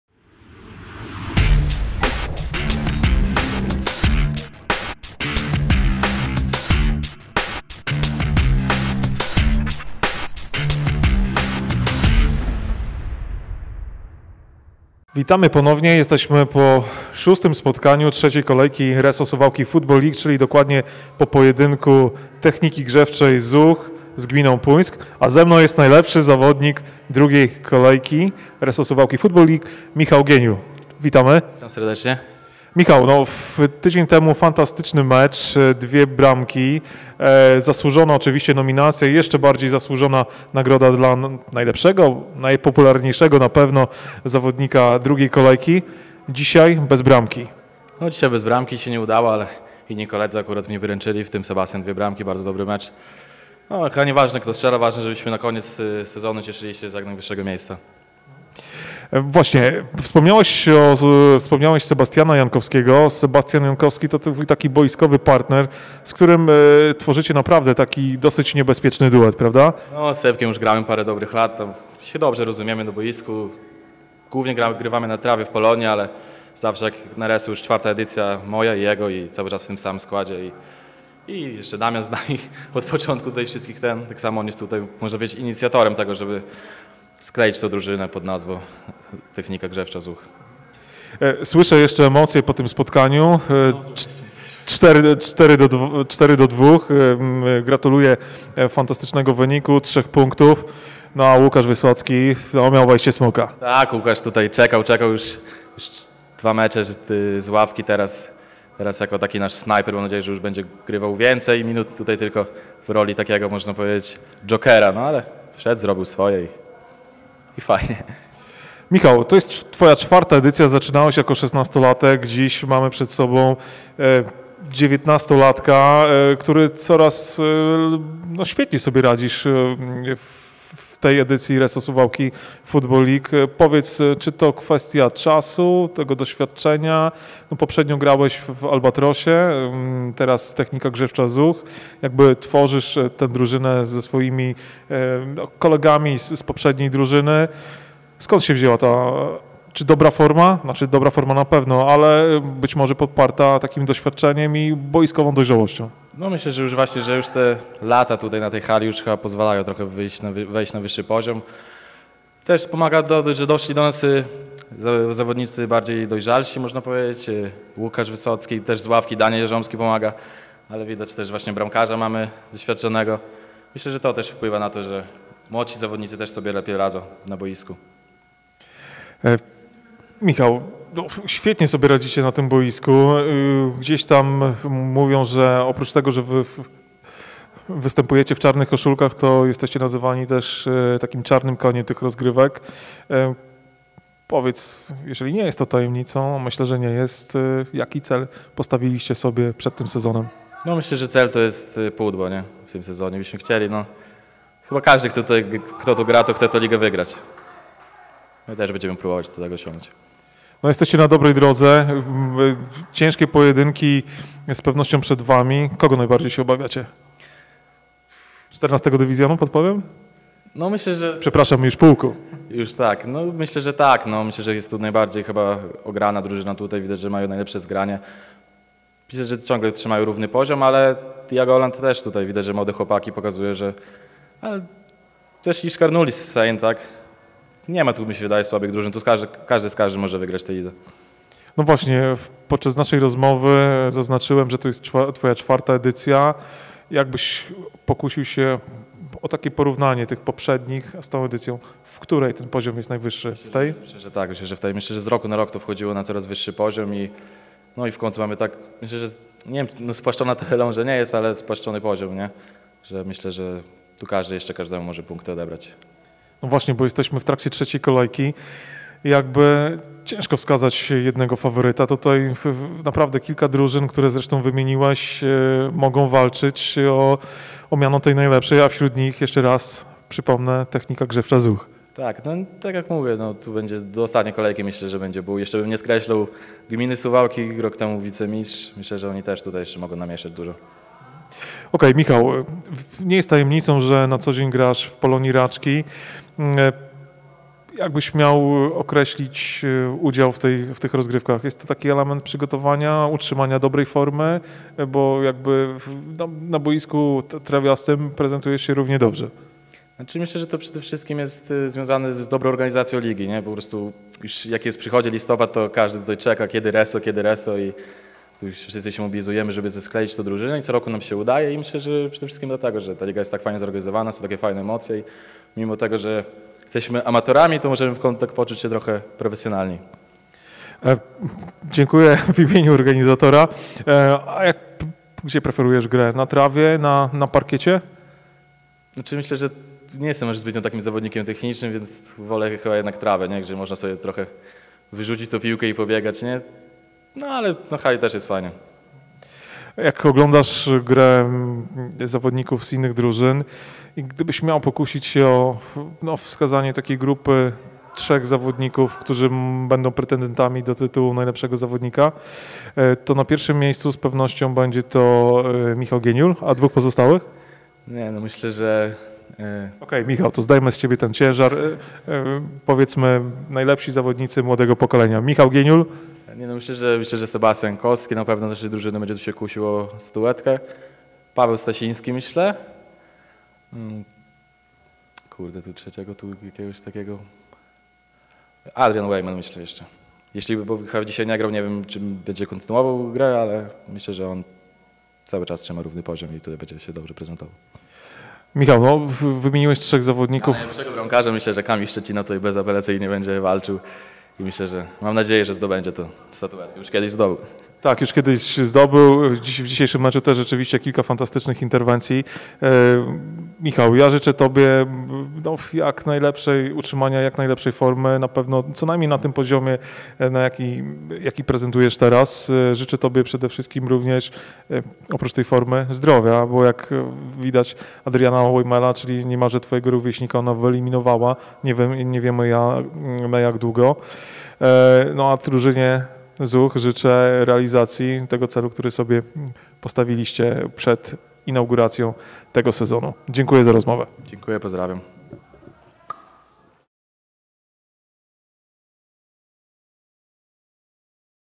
Michał Gieniul - Technika Grzewcza ZUH | RESO Suwałki Football League 2019/2020
Wywiad z Michałem Gieniulem - zawodnikiem Techniki Grzewczej ZUH.

Wywiad został nagrany po III kolejce RSFL: Technika Grzewcza ZUH - Gmina Puńsk 4:2